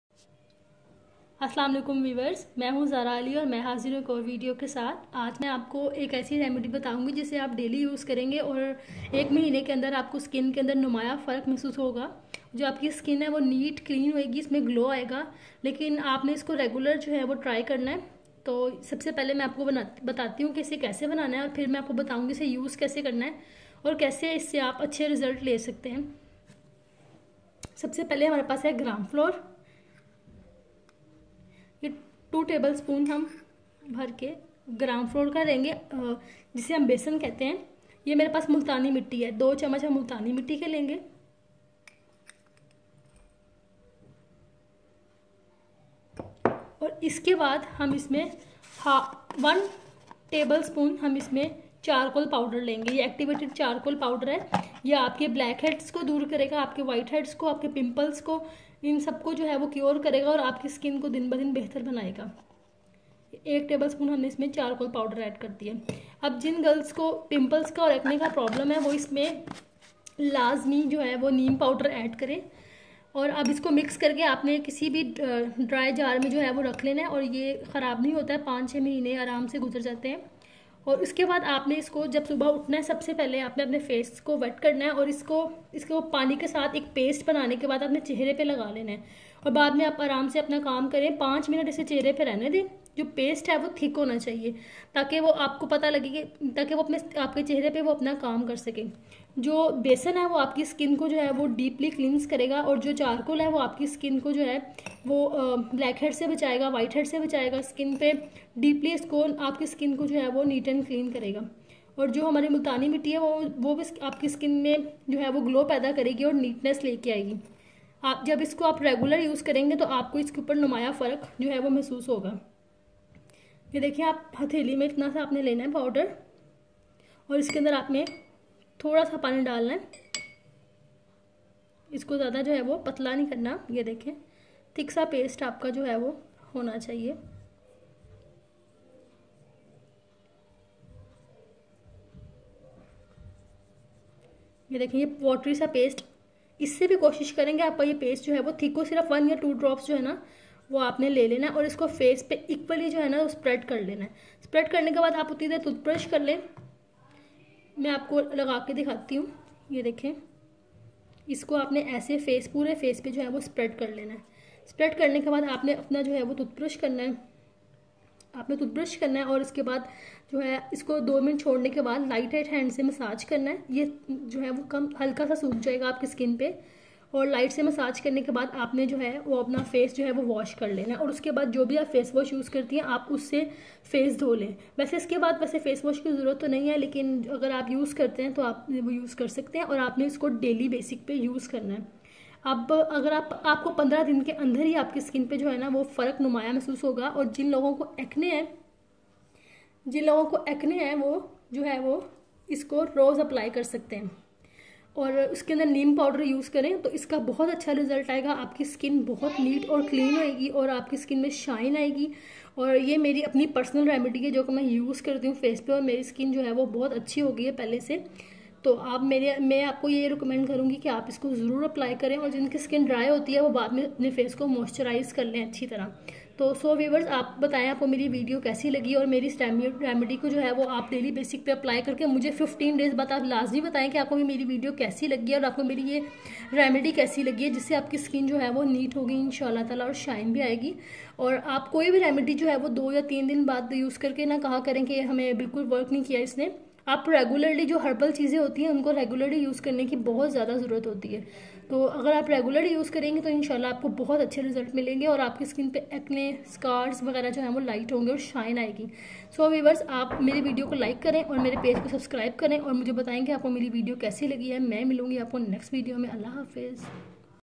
Best deep cleanser for glowing and bright skin
I used besan multani mitti and charcoal powder.it's give owson result